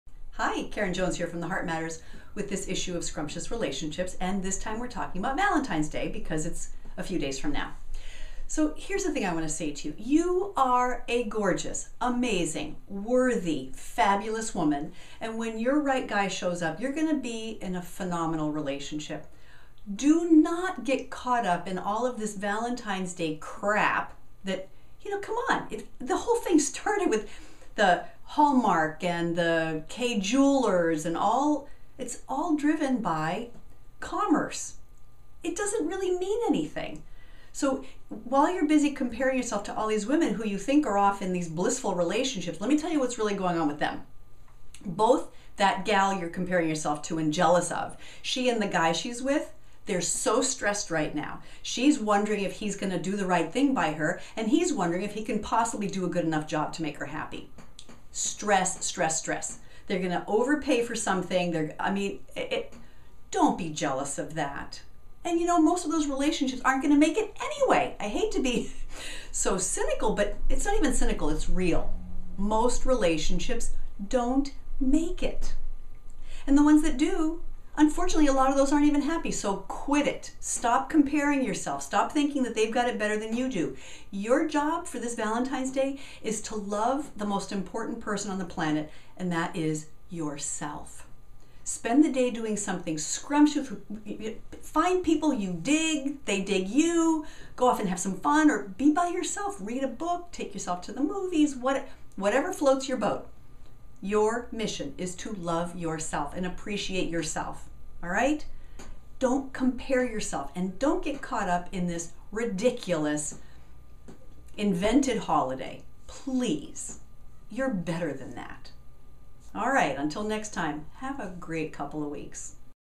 How to Have a Great Valentine's Day as a Single Gal
- It's that time again: Valentine's Day.  Where way too many gorgeous, wonderful women who are totally lovable suddenly feel awful, and compare themselves to the women who have dates for this Hallmark-driven "holiday".   STOP the MADNESS!  This video helps you figure out how.